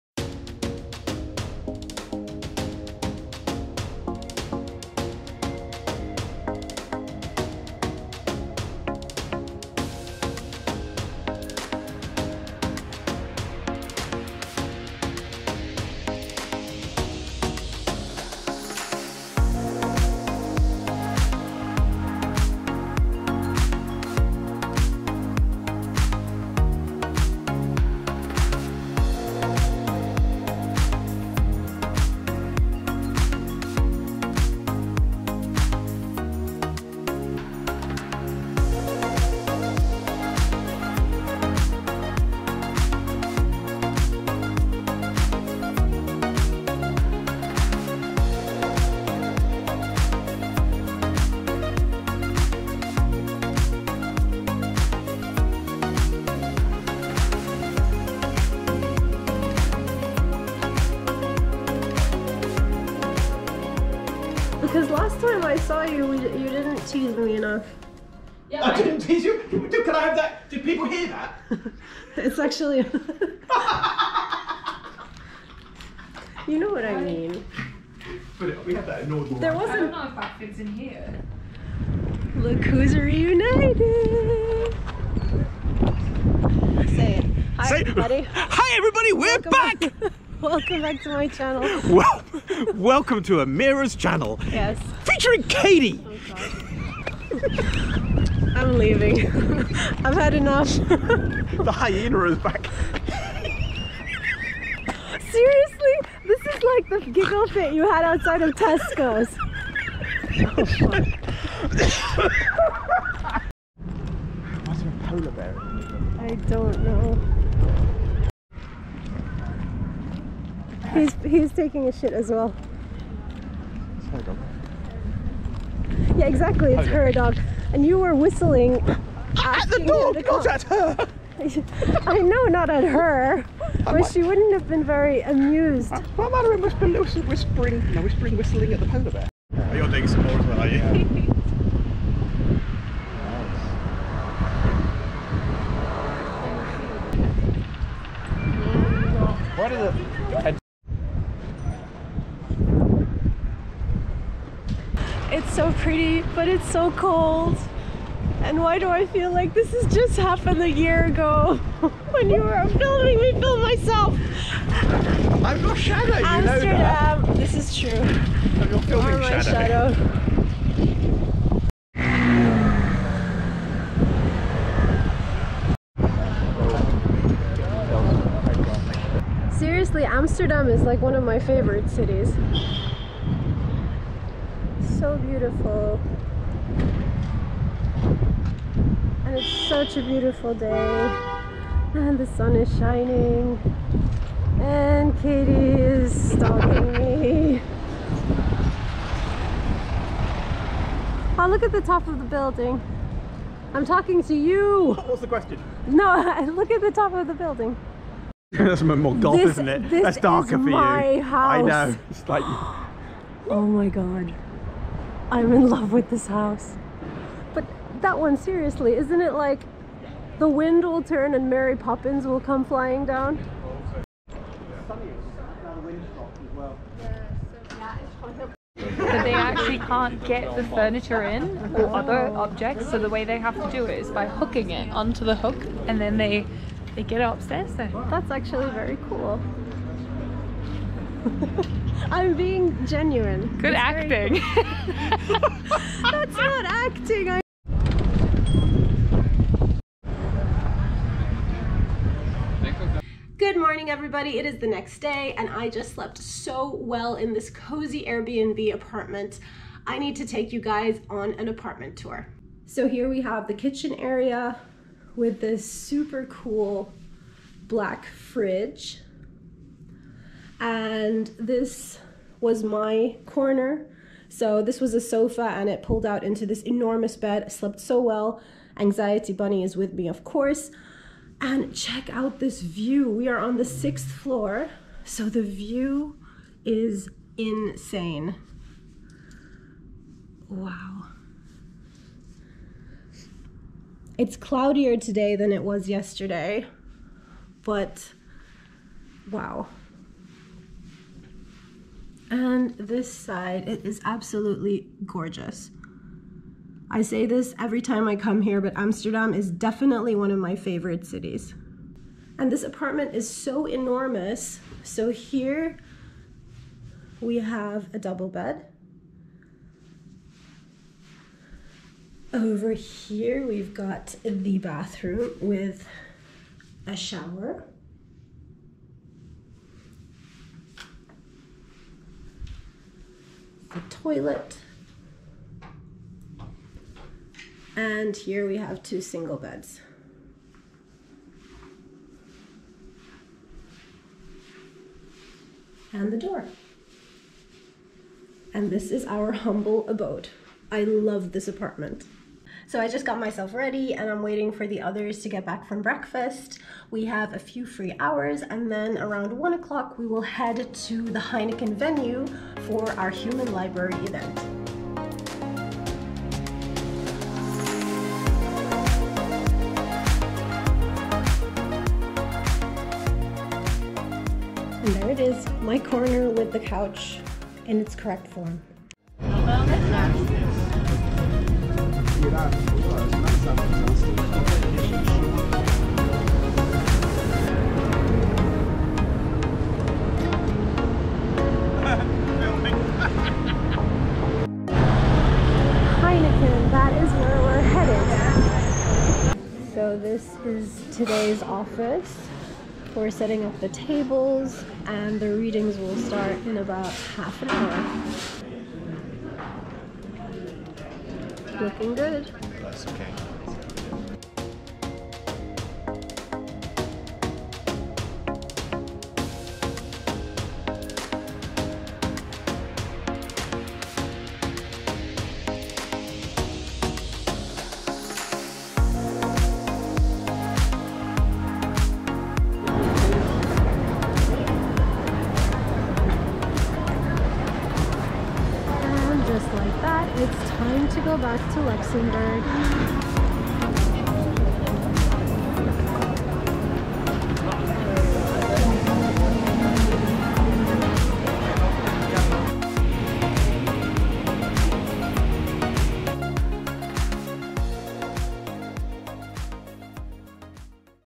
Let's Go To Amsterdam For a Human Library Event: VLOG
Come to Amsterdam with me as I join my friends from the Human Library for an in-person event there. For more information on the Human Library, check out the following links: